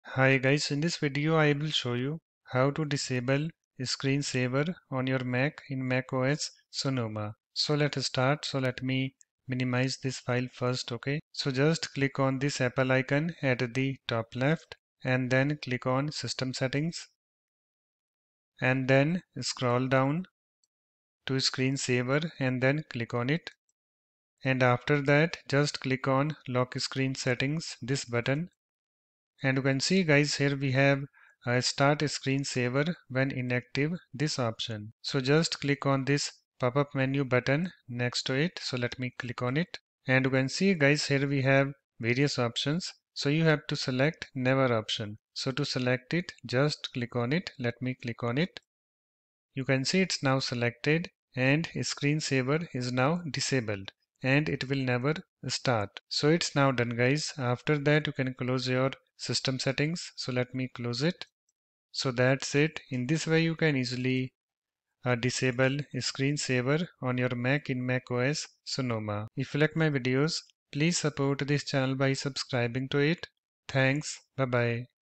How to Disable Screen Saver on Your Mac in macOS Sonoma?
Hi guys, In this Video, I will show you "How to Disable Screen Saver on Your Mac in macOS Sonoma.”